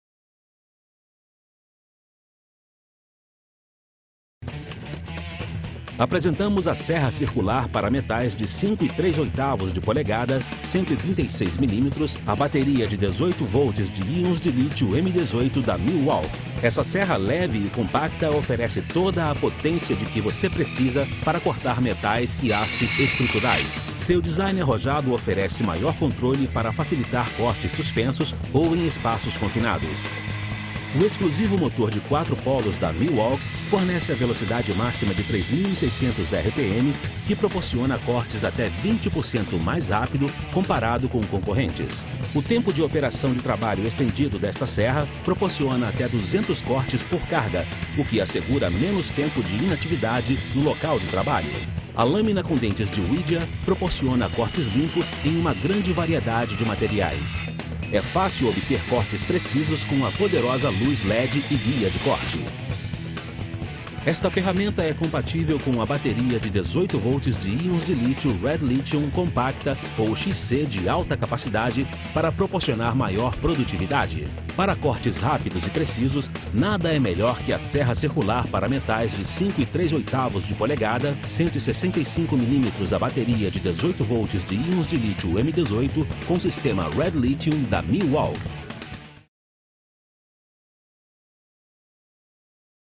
Milwaukee - Serra Circular à Bateria M18
A Serra Circular para Metais à Bateria M18 da Milwaukee é uma ferramenta leve e compacta, que oferece uma grande potência para cortar metais e aços estruturais. Com um design compacto para facilitar cortes suspensos e também em espaços confinados. Seu exclusivo motor 4 polos proporciona cortes 20% mais rápido comparado com os demais concorrentes. As laminas com dentes de wídea proporciona cortes precisos em uma grande variedade de materiais. Essa ferramenta é compatível com bateria 18 volts de íon de lítio REDLITHIUM, possui luz LED e guia de corte.
Uma ótima ferramenta para cortes rápidos e precisos, fácil de usar e de alta qualidade, que irá assegurar uma maior produtividade no seu trabalho.